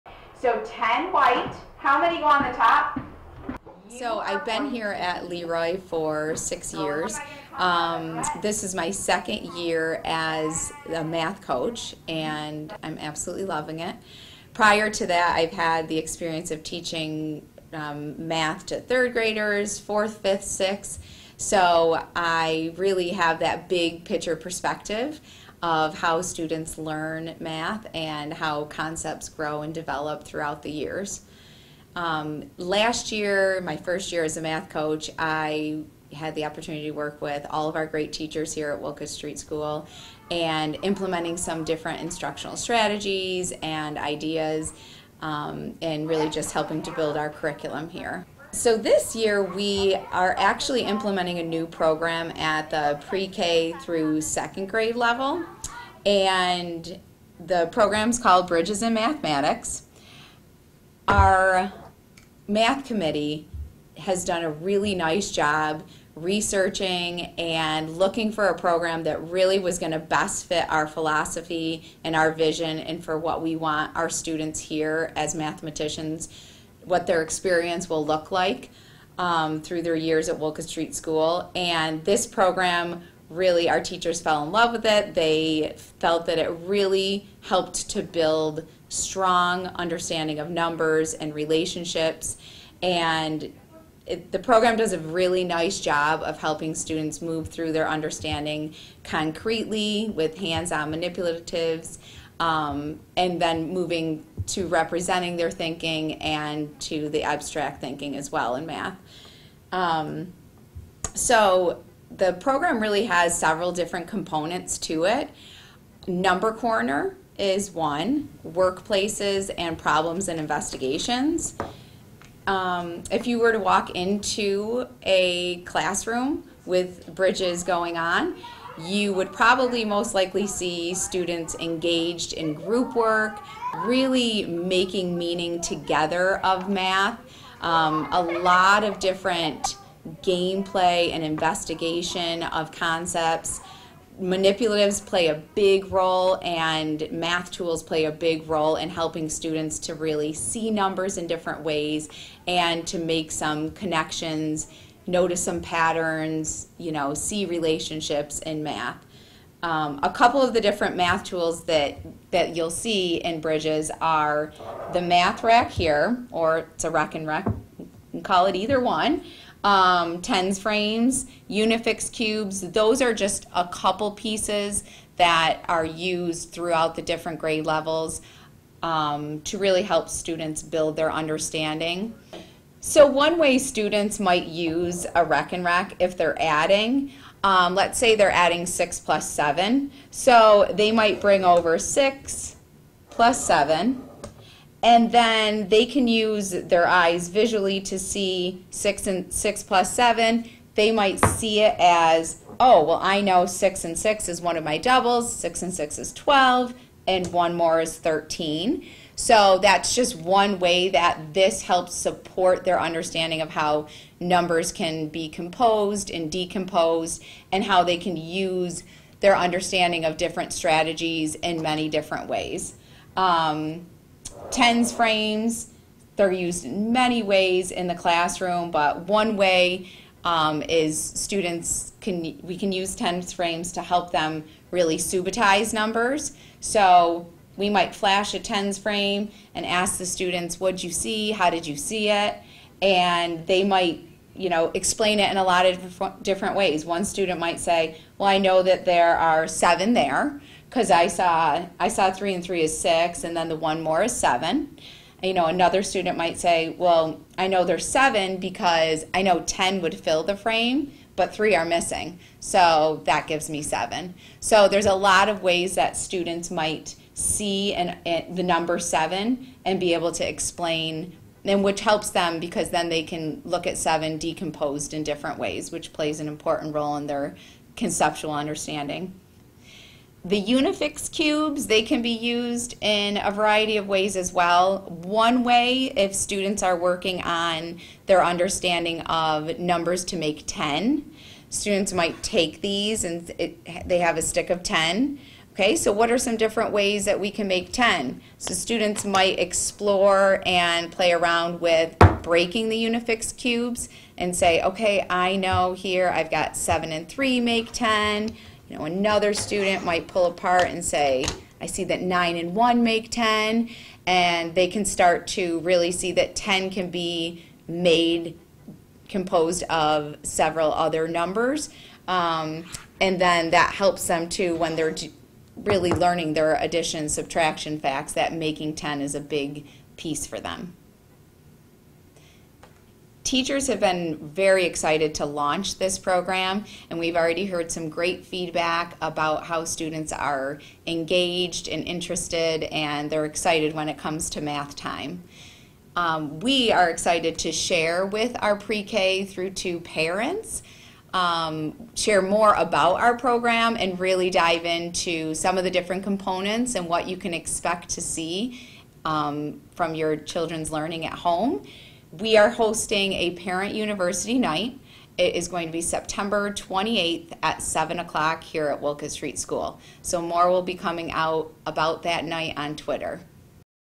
Math Coach Mrs. Wilson explains the new math program for grades Pre-K-2nd
Math Coach Mrs. Wilson explains how grades Pre-K-2nd will be engaging during centers in their classrooms as they learn the new Bridges in Mathematics program at Wolcott Street School.

Parents can learn more about the program during the Parent Math University Night on September 28th at 7:00p.m., in the cafeteria at the Wolcott Street School.